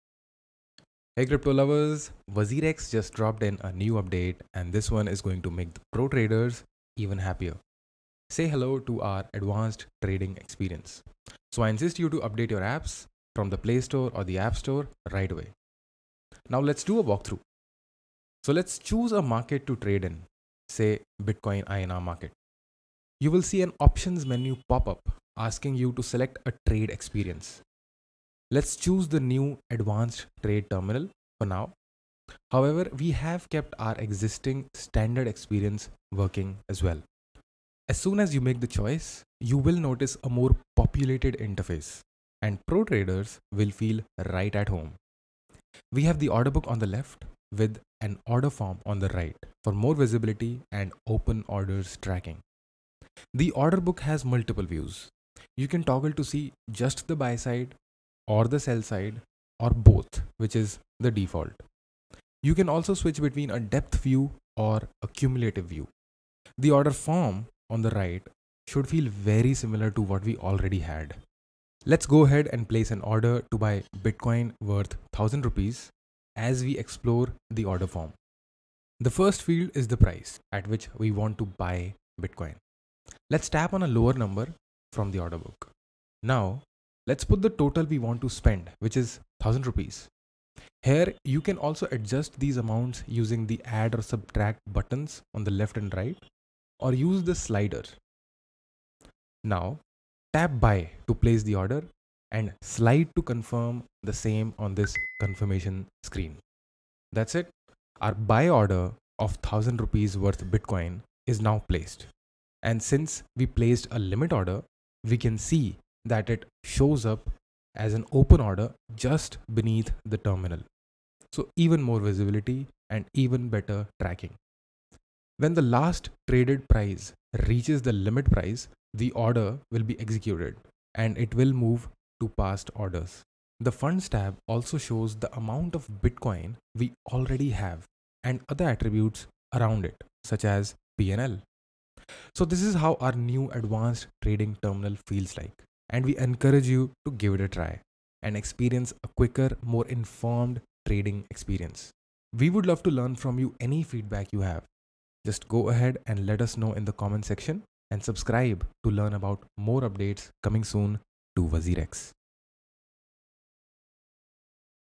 Introducing Advanced Trade Terminal on WazirX App | Step-by-Step Guide
This detailed video tutorial will help you effortlessly navigate the new WazirX interface. It walks you through each step of our enhanced trading platform, showing you how to efficiently use the Order Book, Market Switcher, Open Orders, and Funds Stats.

Keep an eye out and update your WazirX app to experience trading like never before 📲⚡️

*Crypto is an unregulated digital currency, not a legal tender, and subject to market risks. The information provided in this section doesn't represent any investment advice or WazirX's official position.